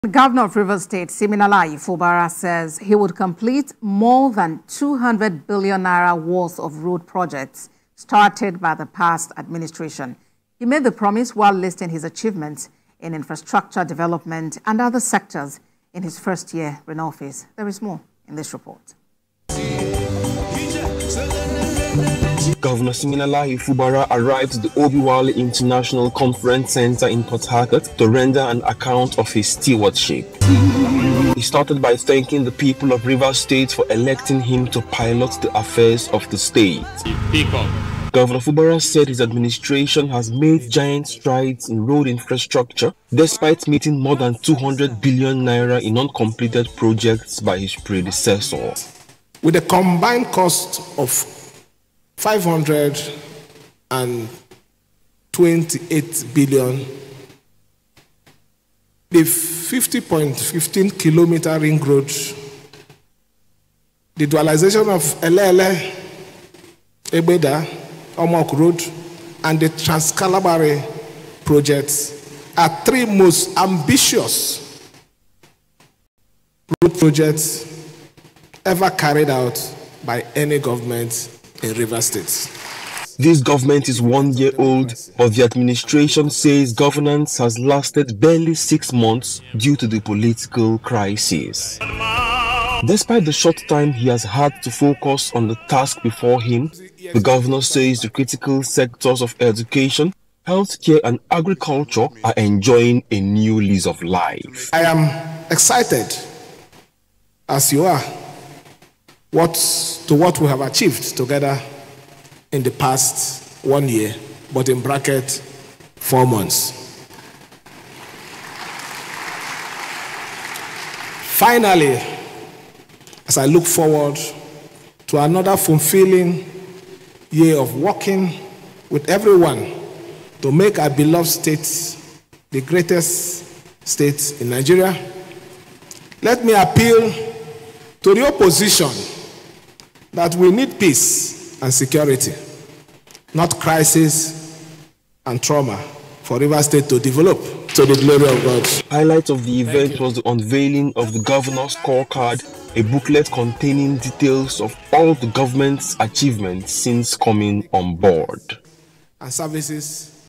Gov. Fubara Lists Nine New Road Projects At N534BN
Governor of Rivers State Siminalayi Fubara says he would complete more than 200 billion naira worth of road projects started by the past administration.
He made the promise while listing his achievements in infrastructure development and other sectors in his first year in office